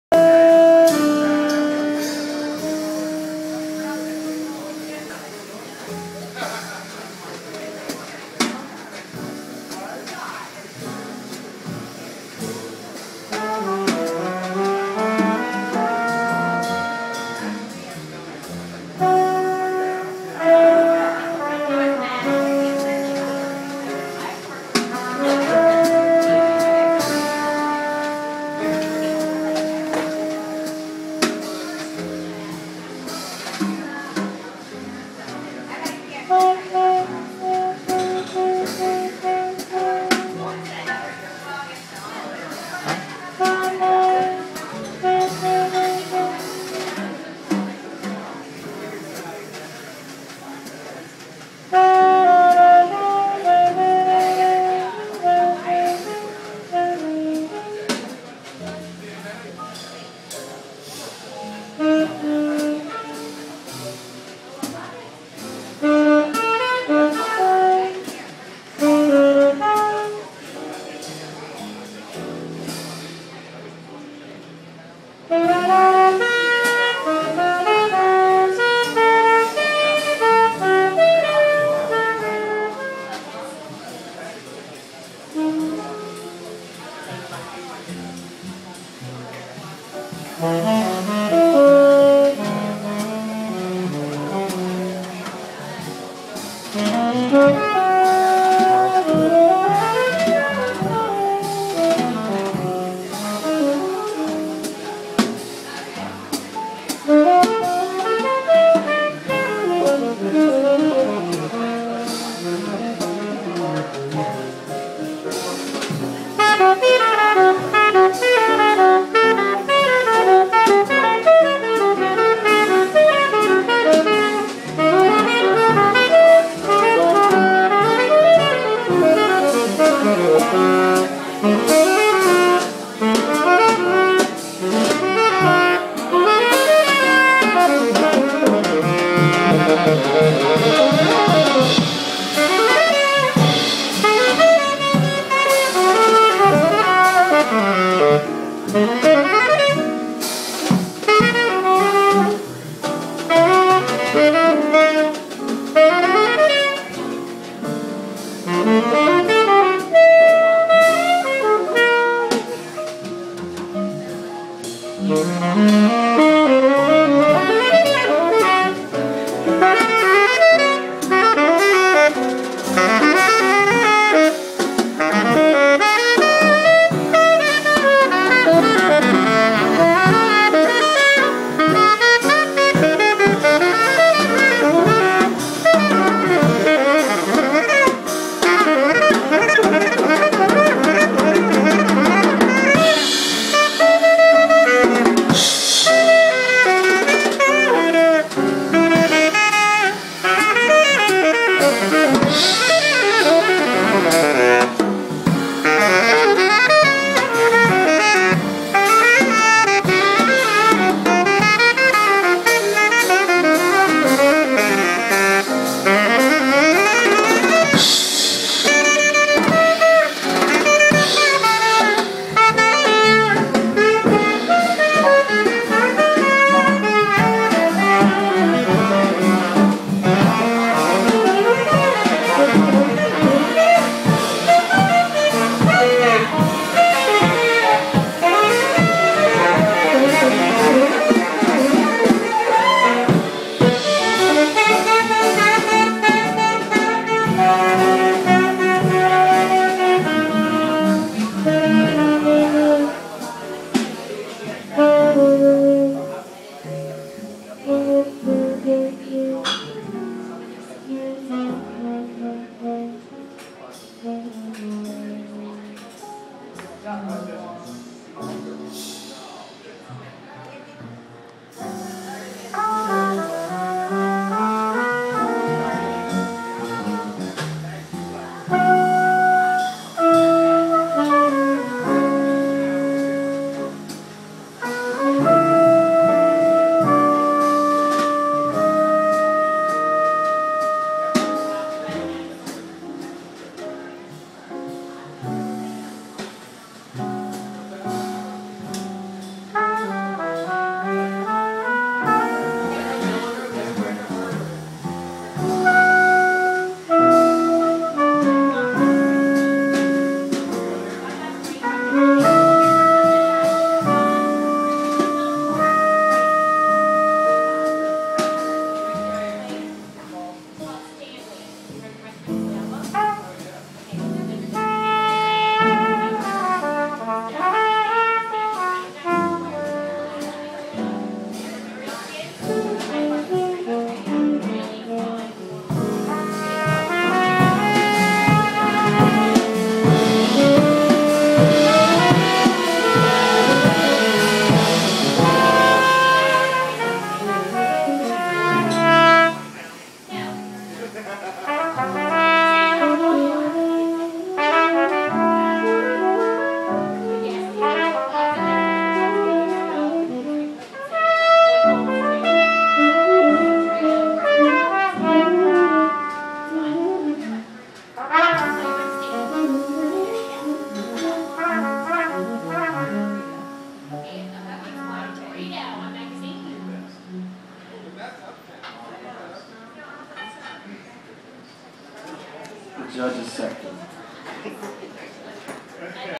The Session Live at The Jazz Playhouse
The Session performing live at Irvin Mayfield's Jazz Playhouse in New Orleans